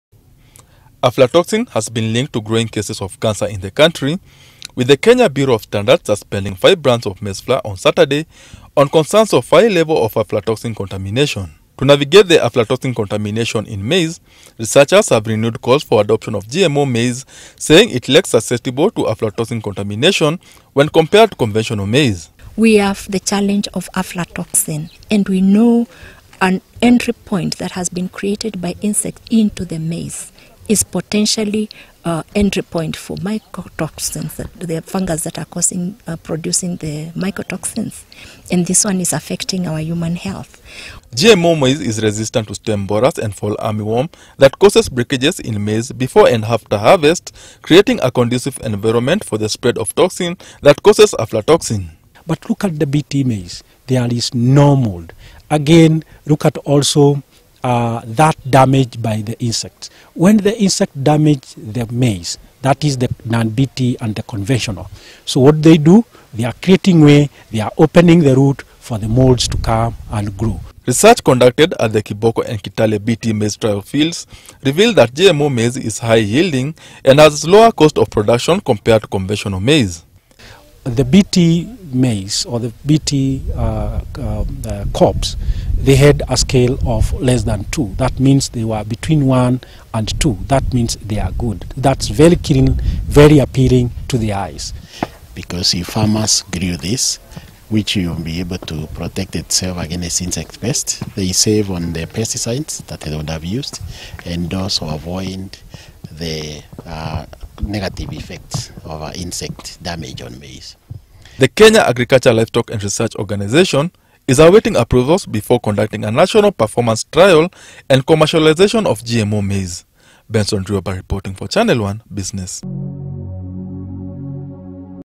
Renewal calls for adoption of GMO maize to contain rise of aflatoxin contamination
Researchers have renewed calls for adoption of GMO maize to contain the rise of aflatoxin contamination. Regina Tende, a maize breeder at Kenya Agriculture Livestock and Research Organization says GMO maize is resistant to pests that causes breakages in maize creating a breeding ground for the fungi that causes aflatoxin.